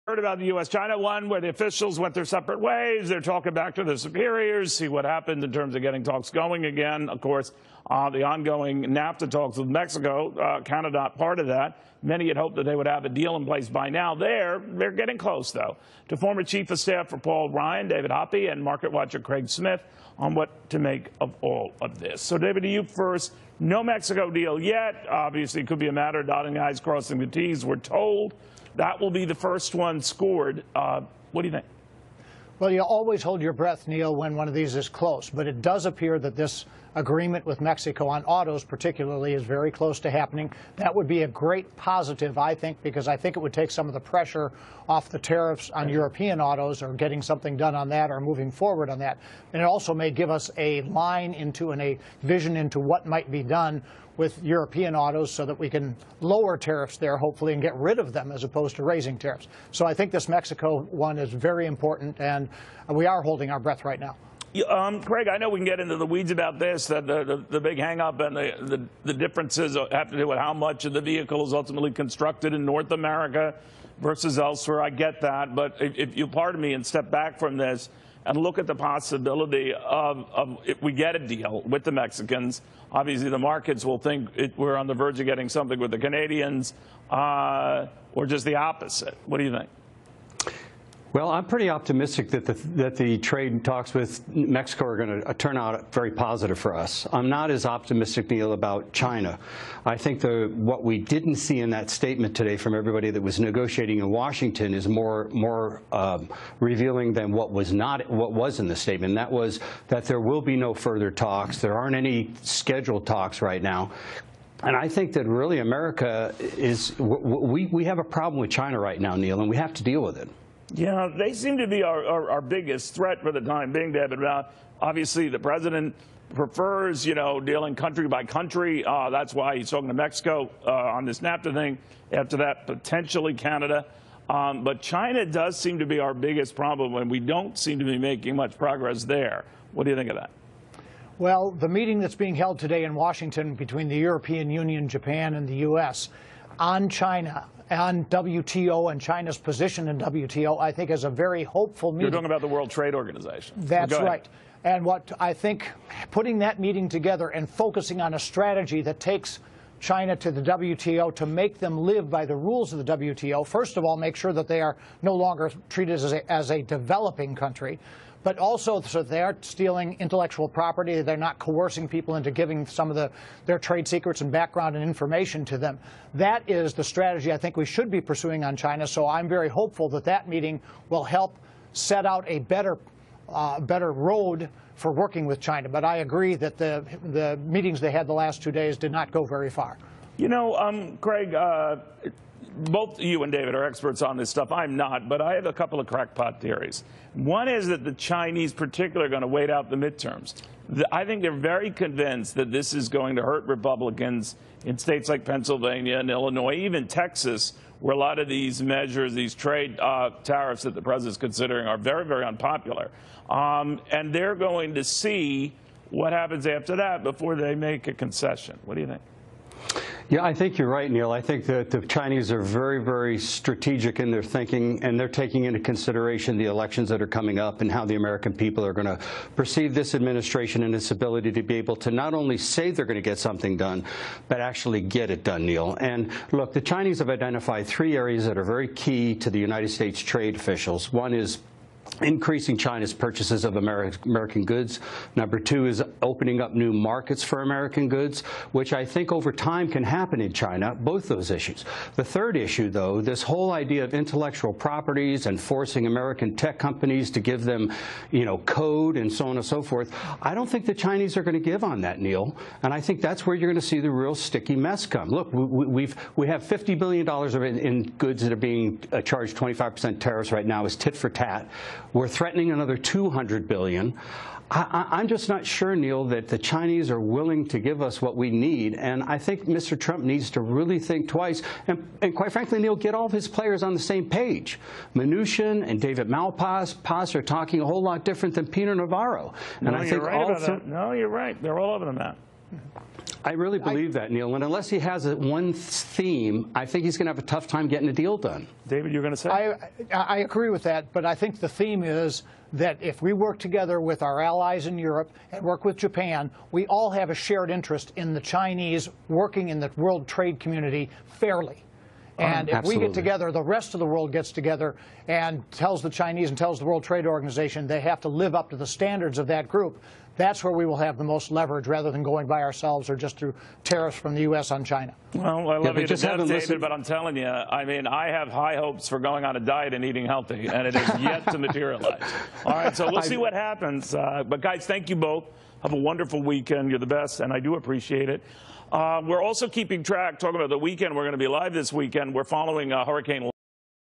China Tariff Talks: Uptrend or Breakdown? -Fox Business
Swiss America Chairman Craig R. Smith joined analyst David Hoppe in discussing the present situation of international U.S. trade deals. Will positive steps toward trade reforms with the European Union, Mexico and Canada help the U.S. make progress on the stalled China tariff talks? Watch to find out.